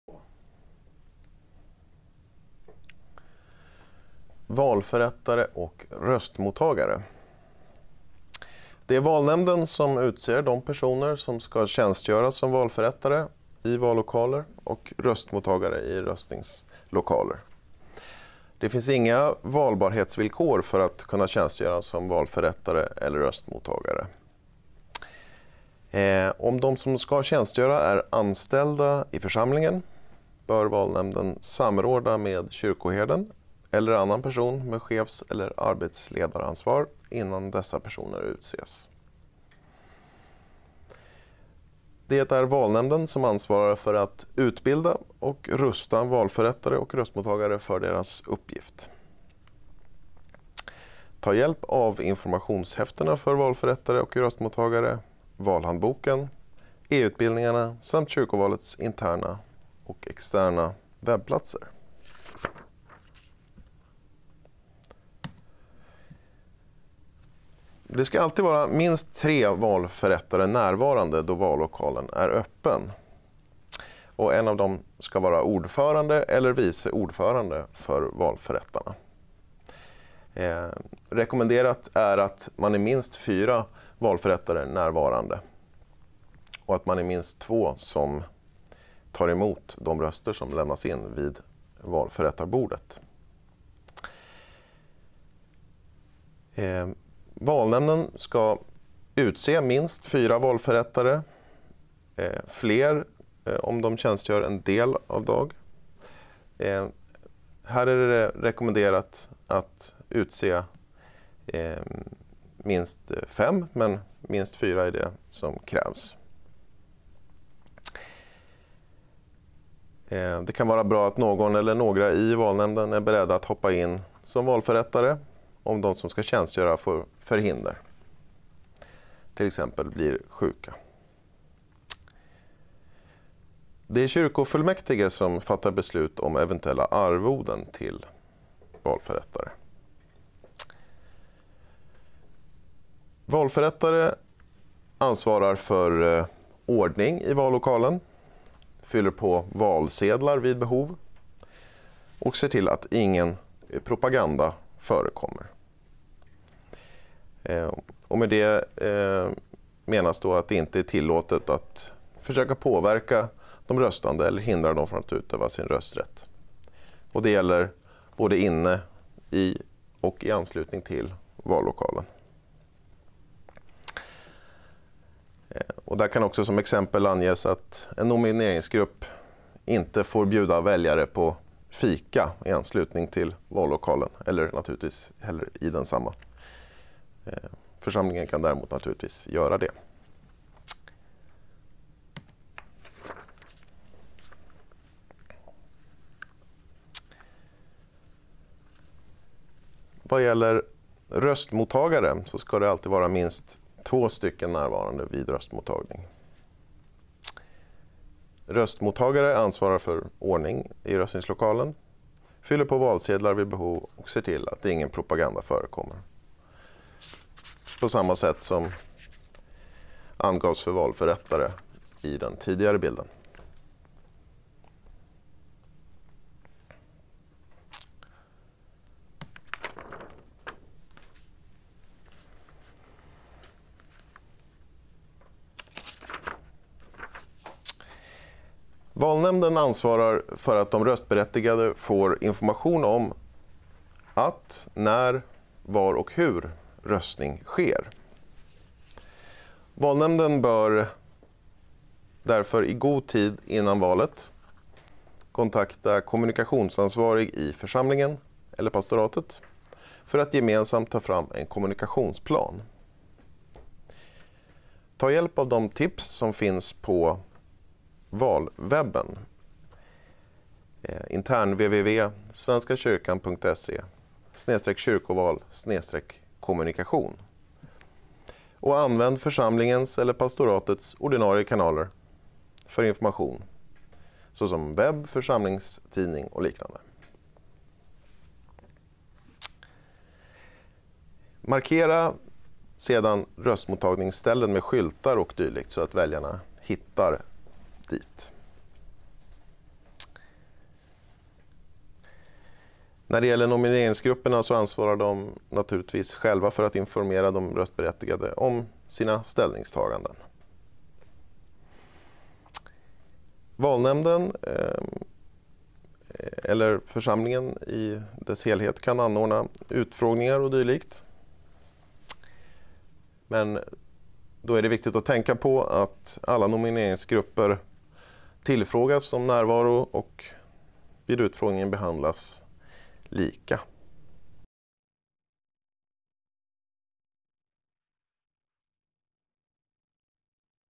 Valnämndsutbildning - 3. Valförrättare och röstmottagare
Denna del handlar om valförrättare och röstmottagare, antal, ordförande och vice ordförande samt om valnämndens särskilda ansvar.